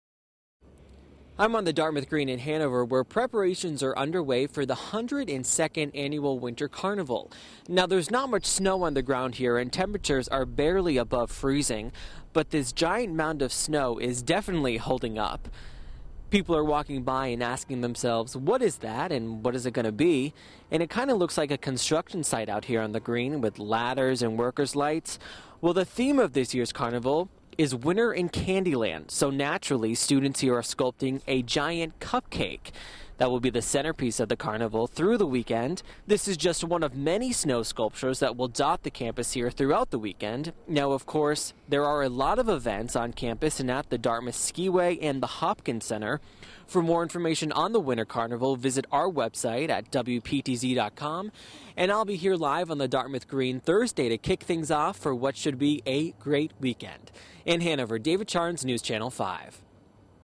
Dartmouth's Winter Carnival Days Away
Preparations are under way at Dartmouth College ahead of the 102nd Winter Carnival.